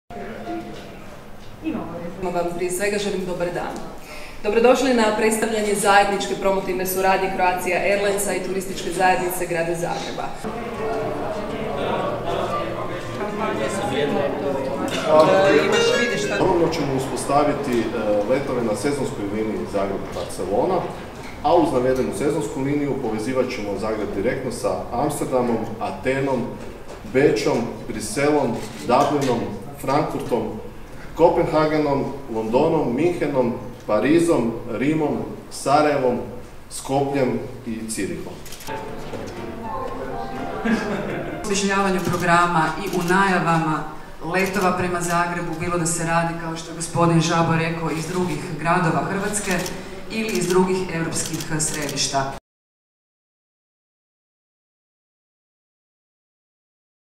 Konferencija za medije o suradnji Croatia Airlinesa i Turističke zajednice Grada Zagreba
U zagrebačkoj poslovnici avioprijevoznika Croatia Airlines održana je konferencija za medije o suradnji s Turističkom zajednicom Grada Zagreba. Na konferenciji su govorili direktor komercijalnih poslova Croatia Airlinesa Slaven Zabo i direktorica Turističke zajednice Grada Zagreba Martina Bienenfeld.